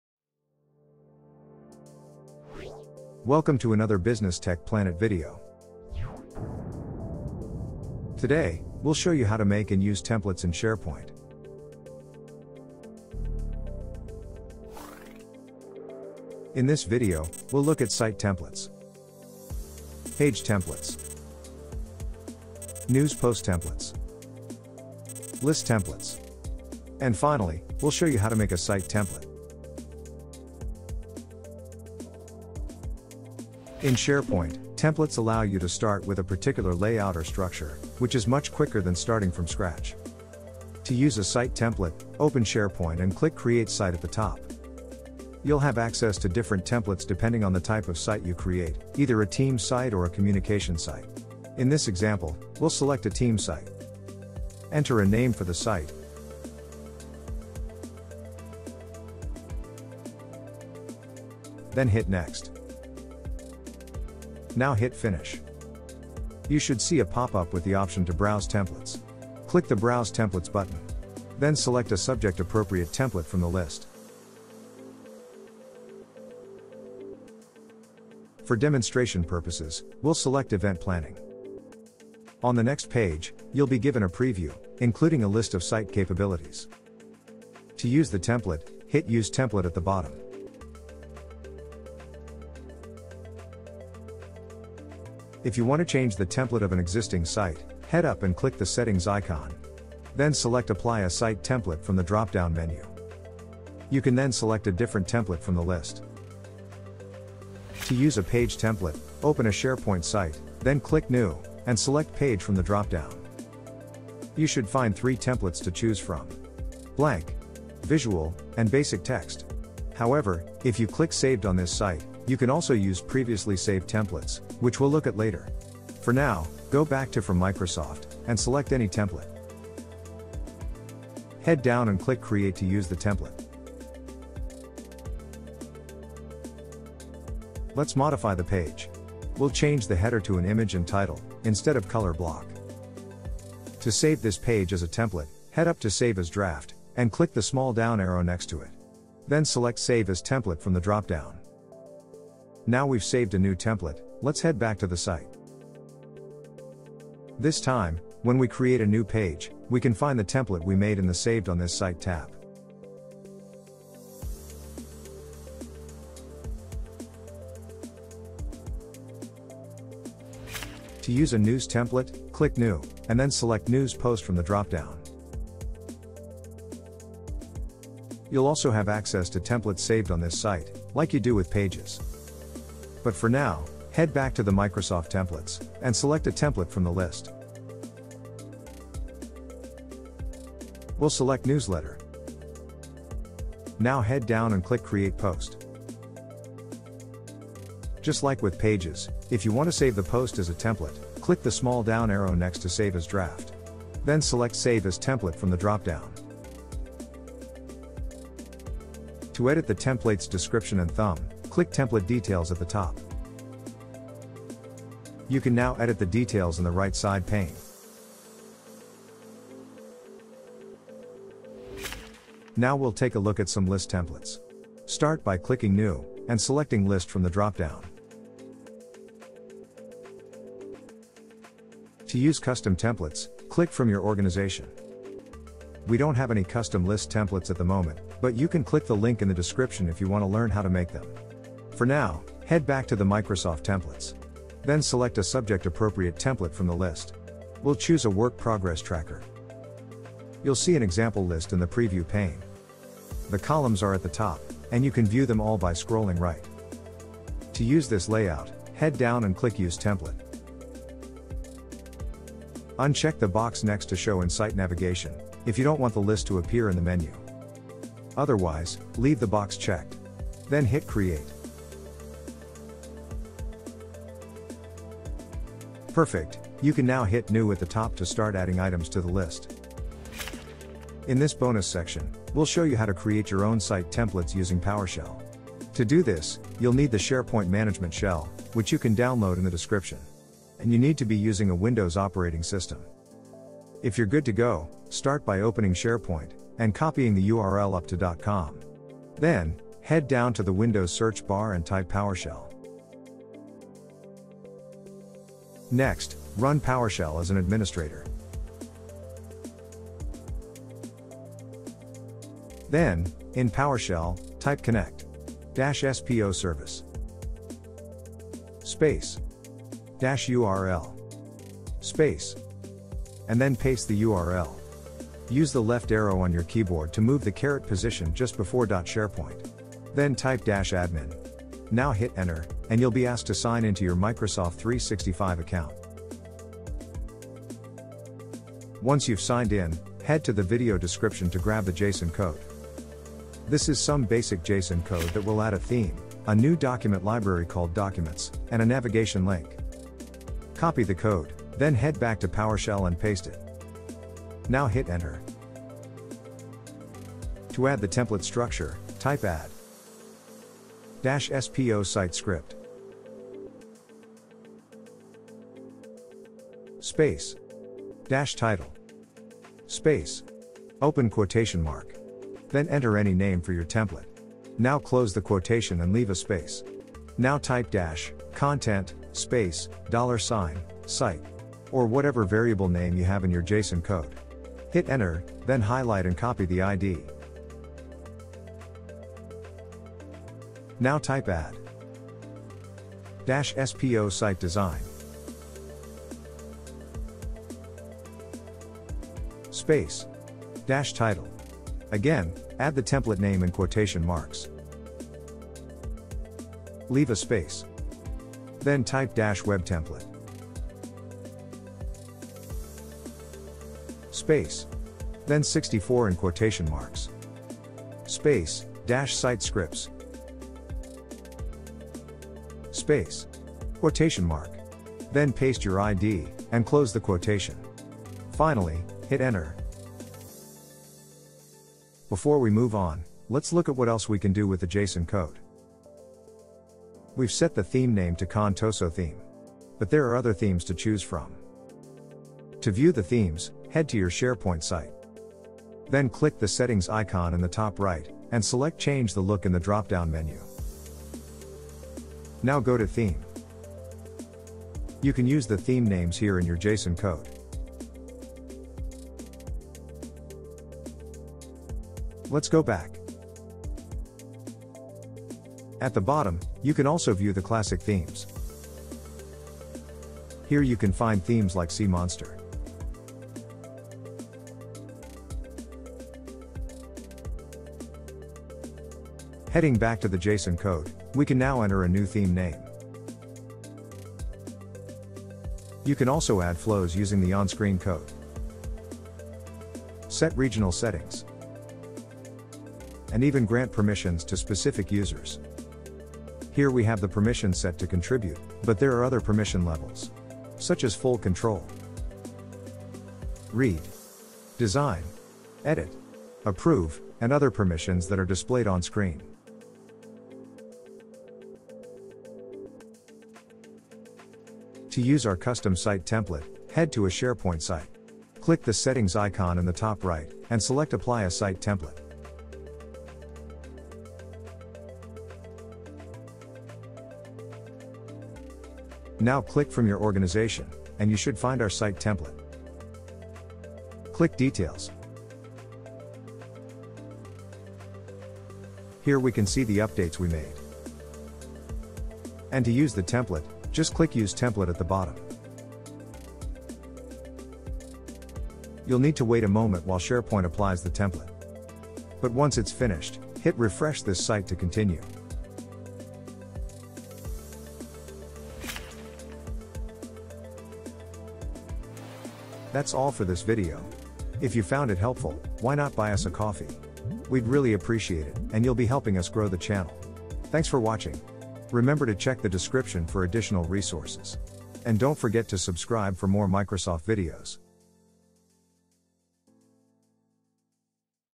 SharePoint Templates: How to make and use them!
In today's video, we'll be looking at SharePoint templates, specifically site, page, news post, and list templates. We also have a bonus method at the end where you can learn how to make your own site templates using PowerShell.

2) Paste JSON code

$site = '
  "actions": [
      "verb": "applyTheme",
      "themeName": "Contoso Theme"
      "verb": "createSPList",
      "listName": "Documents",
      "templateType": 101
      "verb": "addNavLink",
      "displayName": "Custom Link",

3) Add SiteScript
Add-SPOSiteScript -Title "Template Name" -Content $site

4) Add SiteDesign
Add-SPOSiteDesign -Title "Template Name" -WebTemplate "64" -SiteScripts "site-script-id"

🕐 Timestamps:

0:00 - Intro
0:34 - Site Templates
1:48 - Page Templates
2:51 - News Post Templates
3:42 - List Templates
4:41 - Bonus Method - Make a custom Site Template!
9:22 - Outro